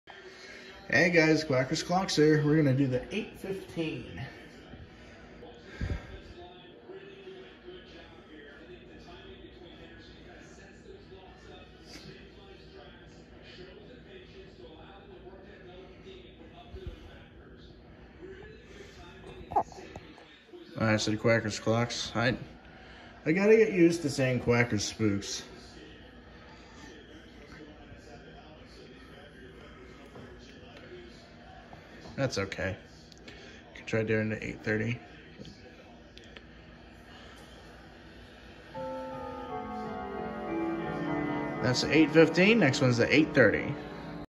8:15PM chime (St. Michael’s) Saturday, September 24, 2022
The band is playing.